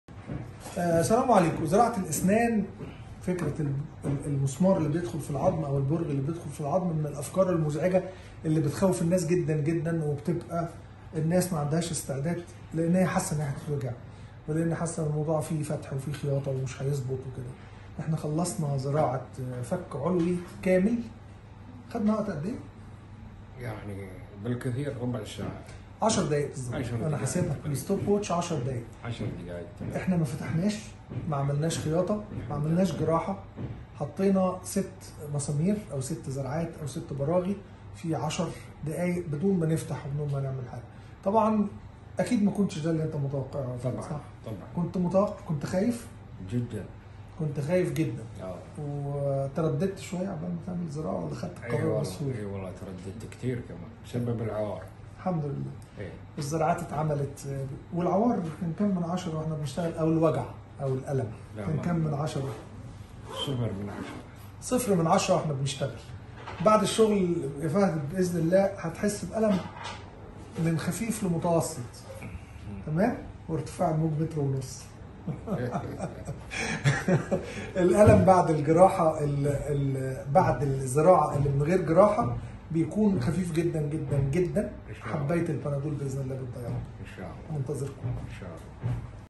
زراعة فك كامل علوى على 6 زرعات فى 10 دقائق
تجربة جديدة مع
لفك علوى كمل على 6 زرعات فى 10 دقائق بدون جراحة معقدة وبدون ألم مع
الدكتور حسام عاشور إستشارى جراحة الوجه والفكين وزراعة الأسنان
نتشرف بزياراتكم
الكويت - مركز اسنانكو 22280801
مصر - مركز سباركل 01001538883